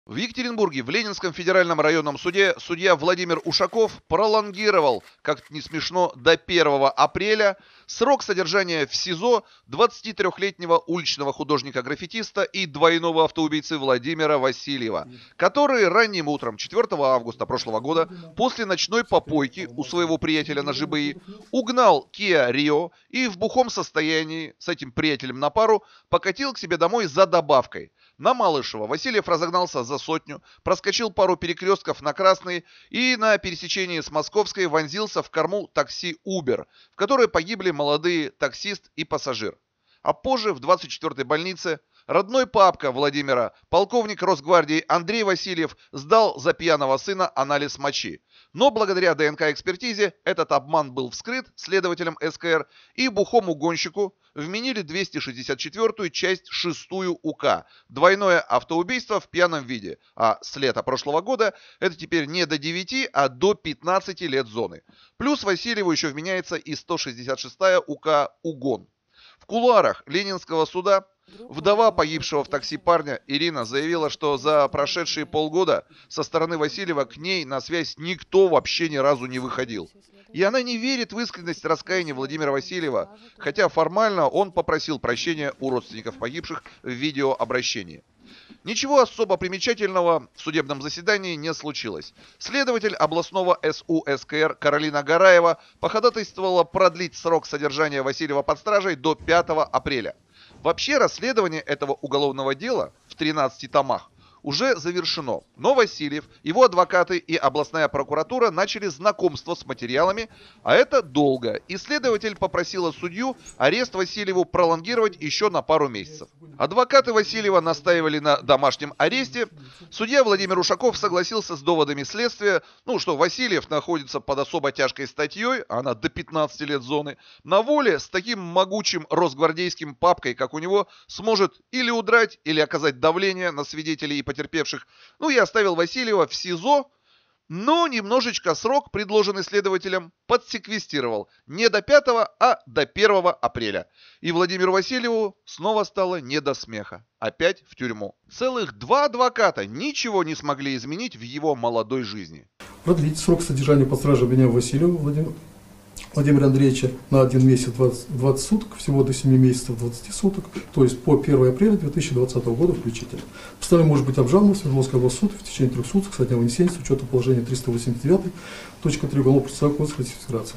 Двойного автоубийцу оставили в СИЗО до 1 апреля. Real video
Расследование СКР завершил, но нужно время на ознакомление с 13 томами дела